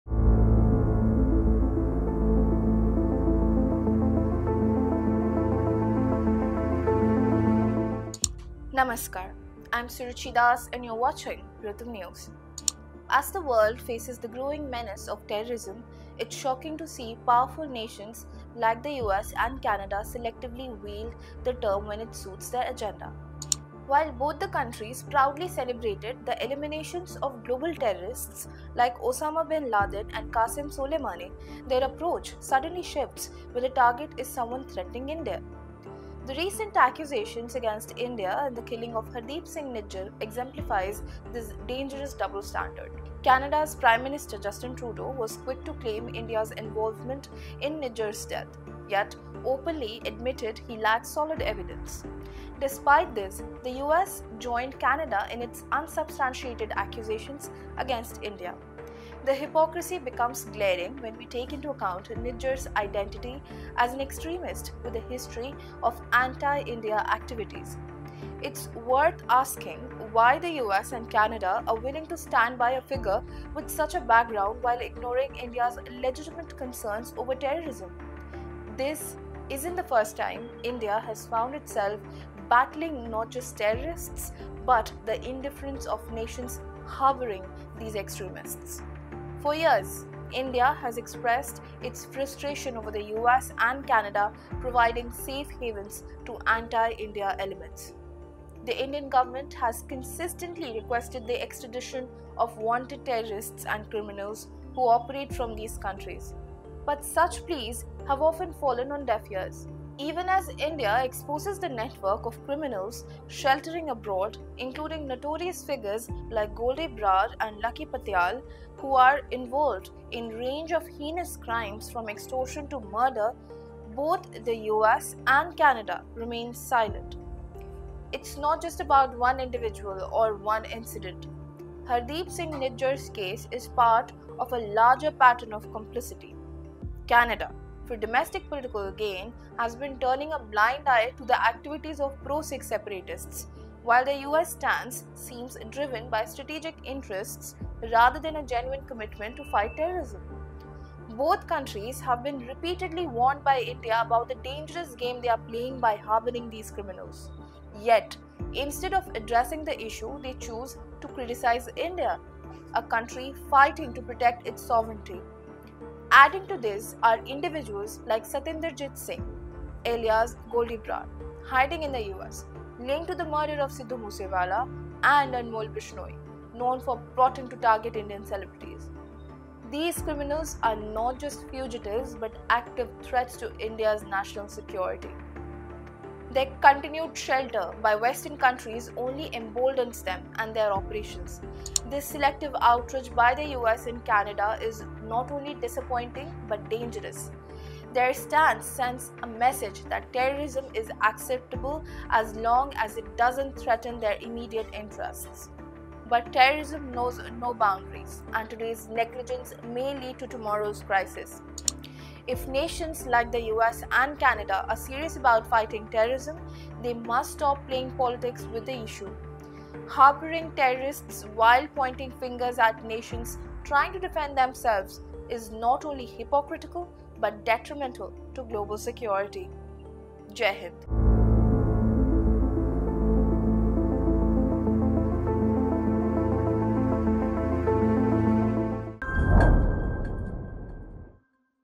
US and Canada’s Hypocrisy: Harboring Anti-India Terrorists While Blaming India for Najjar's Death
As the world faces the growing menace of terrorism, it's shocking to see powerful nations like the US and Canada selectively wield the term when it suits their agenda. While both countries proudly celebrated the eliminations of global terrorists like Osama bin Laden and Qassem Soleimani, their approach suddenly shifts when the target is someone threatening India. The recent accusations against India in the killing of Hardeep Singh Nijjar exemplifies this dangerous double standard.